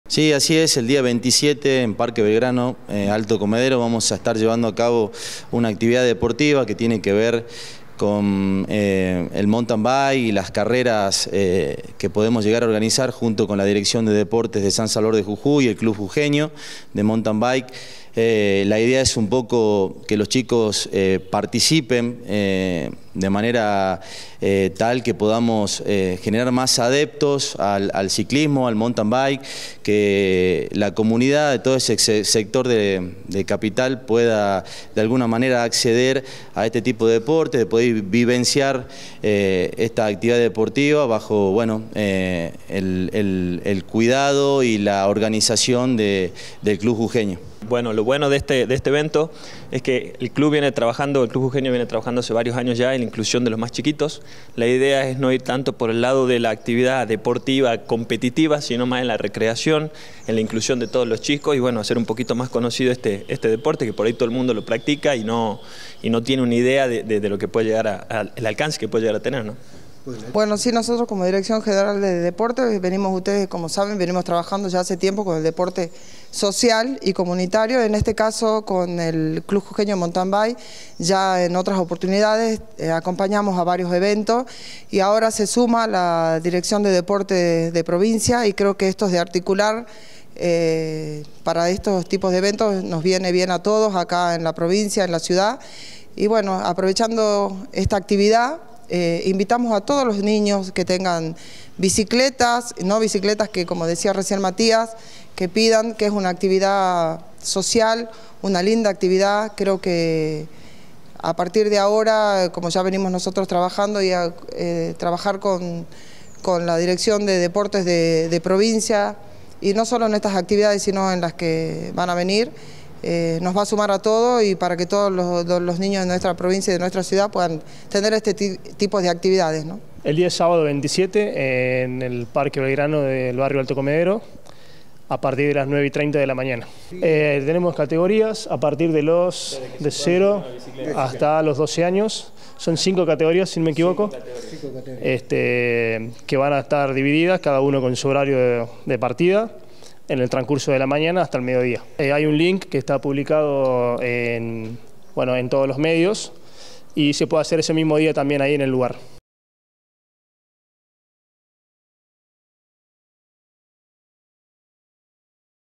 Convocan a la reunión social de “Mountain Bike” infantil en el Parque Gral. Belgrano de A. Comedero
El sábado 27 de enero, a partir de las 9.30 hasta las 12.30 hs., en el Parque Gral. Belgrano de Alto Comedero, el Club Jujeño de Mountain Bike con la colaboración de la Municipalidad de San Salvador de Jujuy y la Secretaría de Deportes de la Provincia, realizarán la jornada de “Carrera infantiles” con la finalidad de promocionar e incluir a la actividad ciclística a niños de 5 a 12 años.